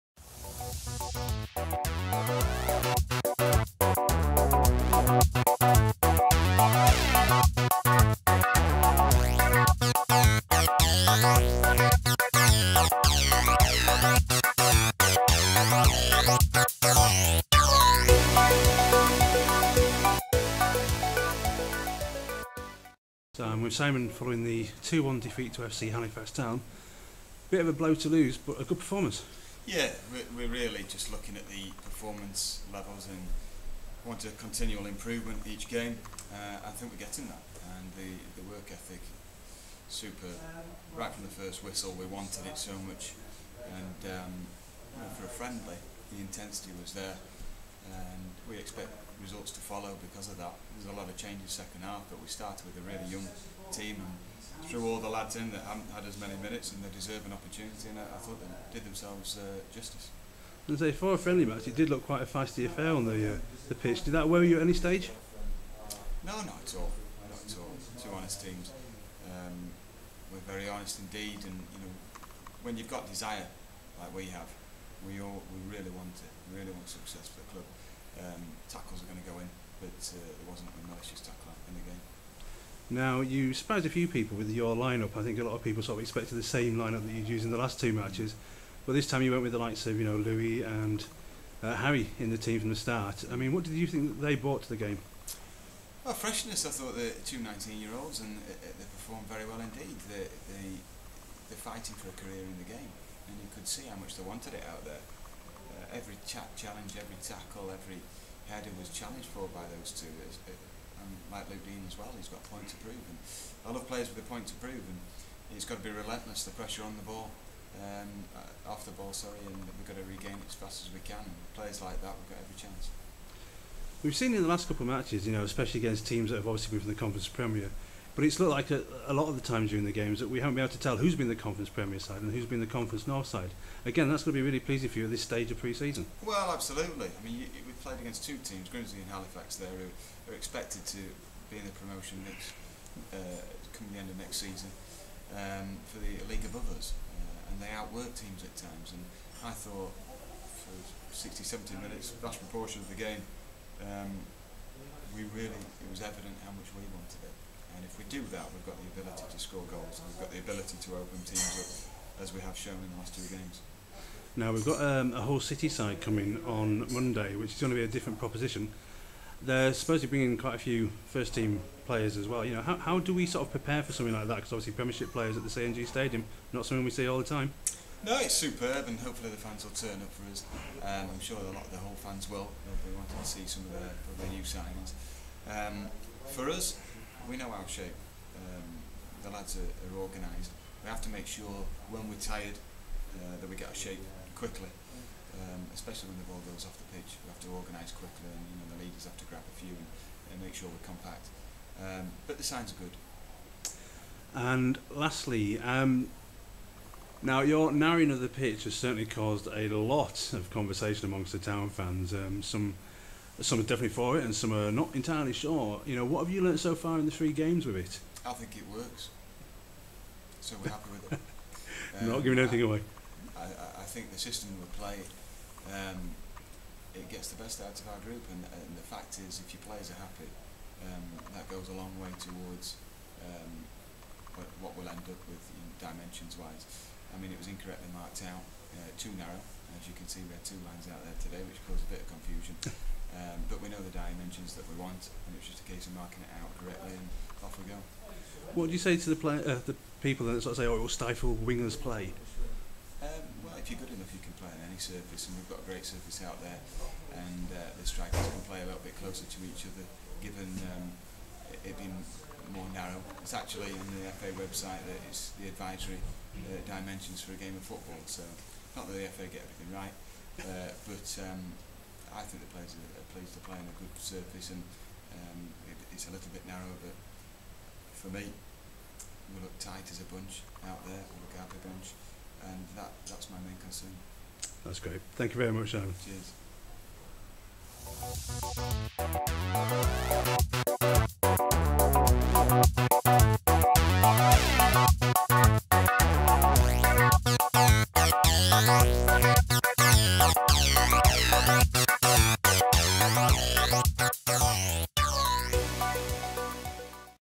Simon Weaver - FC Halifax Town 19/07/2014
Harrogate Town's manager Simon Weaver spoke to Press Officer Peter Arnett following the 2-1 pre-season friendly defeat against FC Halifax Town.